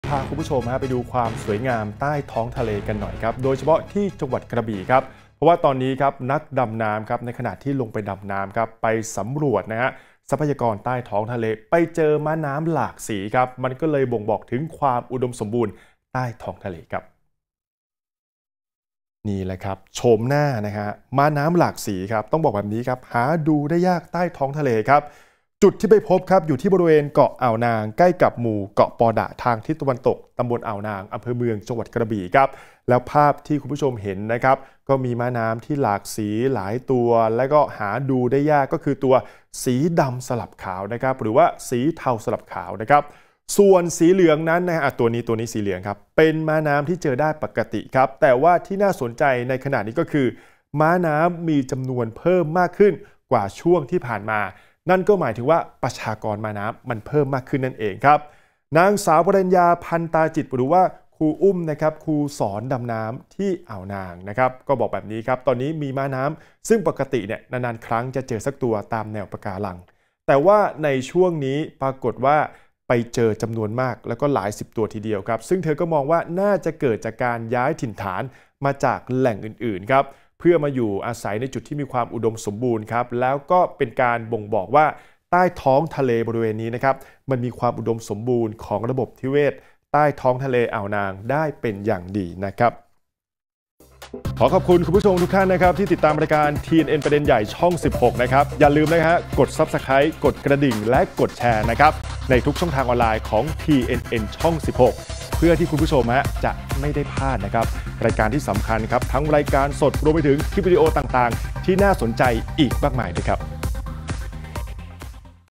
หาดูยากพบม้าน้ำหลากสี ใต้ทะเลอ่าวนาง จ.กระบี่ l TNNประเด็นใหญ่ 14-09-2565
พบม้าน้ำหลากสี แหวกว่ายใต้ทะเลอ่าวนาง นักดำน้ำเผยมีจำนวนเพิ่มมากขึ้น ชี้ถึงความสมบูรณ์